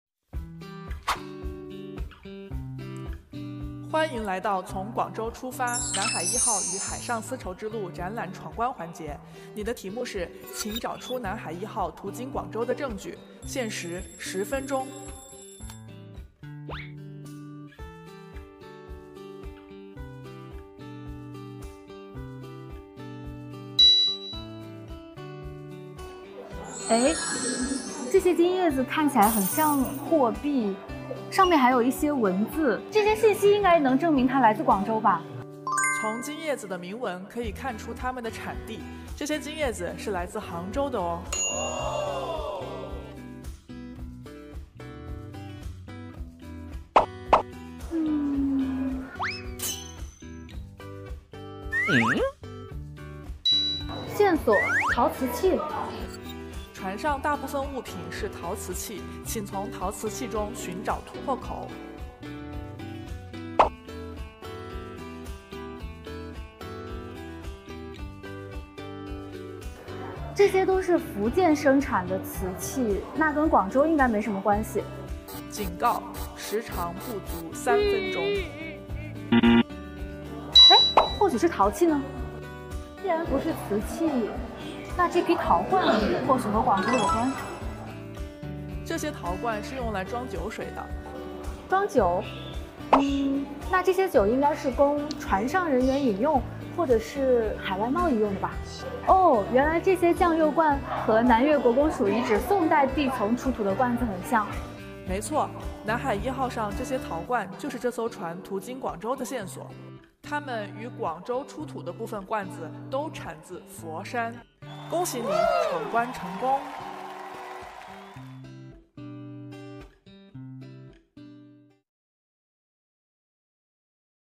Let's solve the mysteries of Nanhai I Shipwreck Maze
On this fully loaded cargo ship from the Southern Song Dynasty, what items can prove that it had been to Guangzhou? If anyone gets lost in the Nanhai I Shipwreck Maze, how can they decipher the mysteries? Recently, the exhibition "Starting from Guangzhou: Nanhai I Shipwreck and the Maritime Silk Road" opened in the King Tomb Exhibition Area of the Nanyue King Museum in Guangzhou. Geniuses must find evidence of the Nanhai I Ship passing through Guangzhou within 10 minutes at this exhibition.